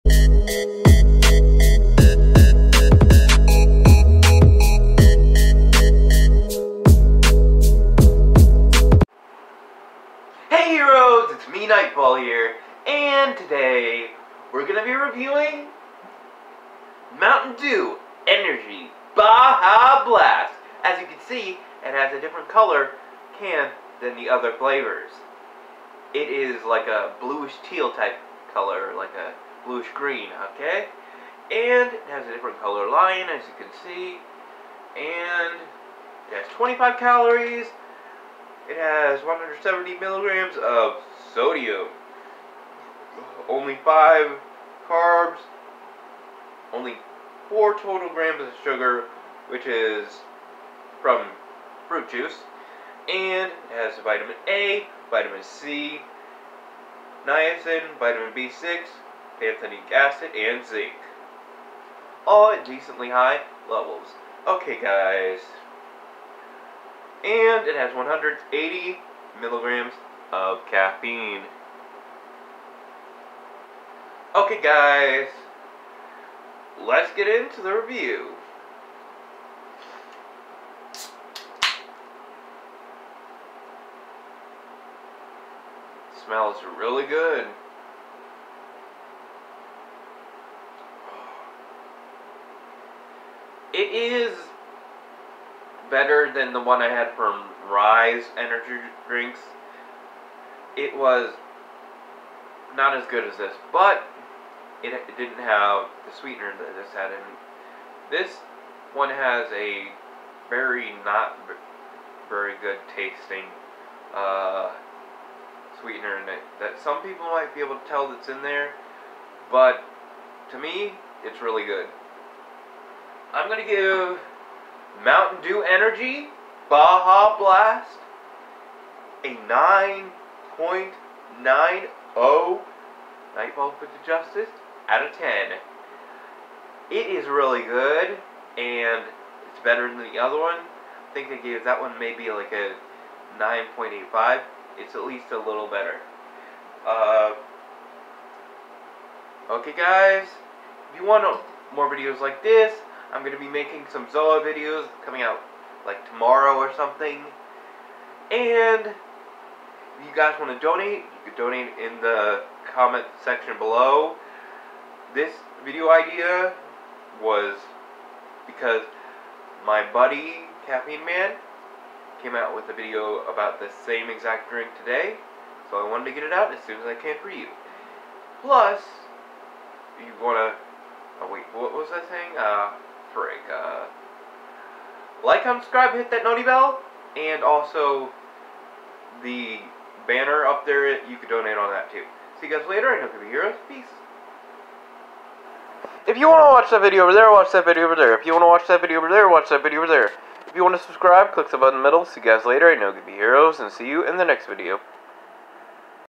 MTN Dew Energy Baja Blast Energy Drink Review
Today I'm Reviewing The New Baja Blast Flavor